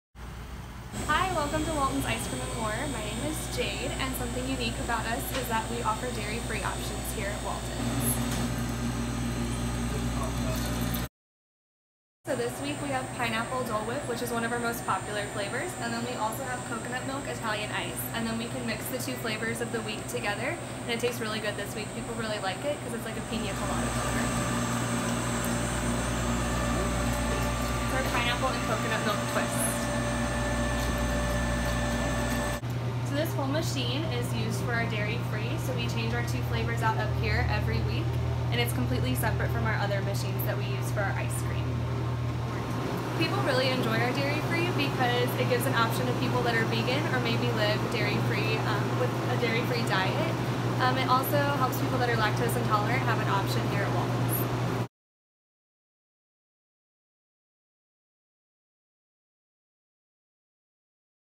Smithton, IL, ice cream place serves dairy-free soft serve
Dairy-free ice cream is now available at Walton’s Ice Cream and More in Smithton, Illinois, so that those kids, teens, and adults who are lactose intolerant can still enjoy the dessert food.

Video by Cara Anthony/News-Democrat